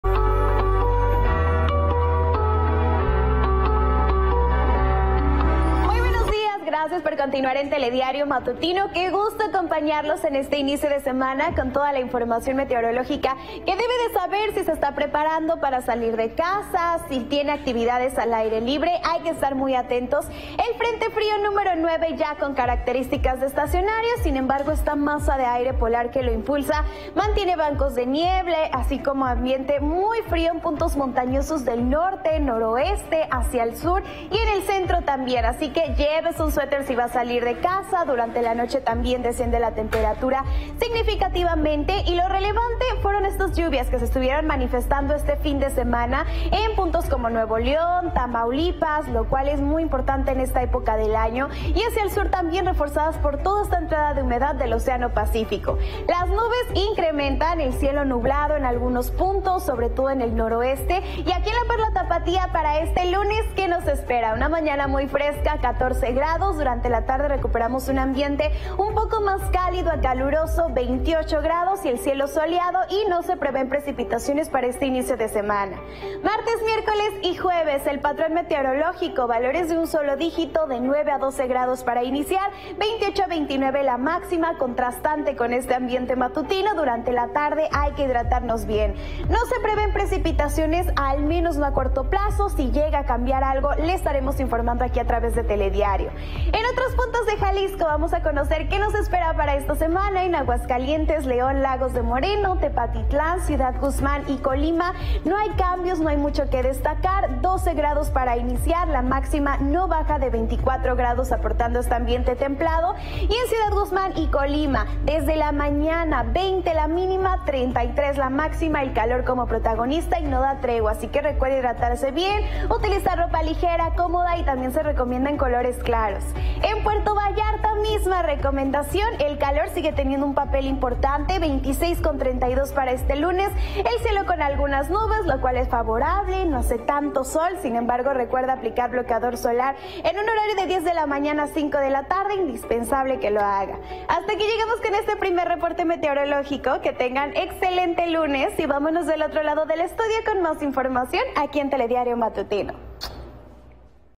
Pronóstico del tiempo HOY 13 de noviembre de 2023 en Jalisco
Checa el clima que le depara a Guadalajara para este inicio de semana. ¿Hará frío? Aquí te decimos.

TELEDIARIO GUADALAJARA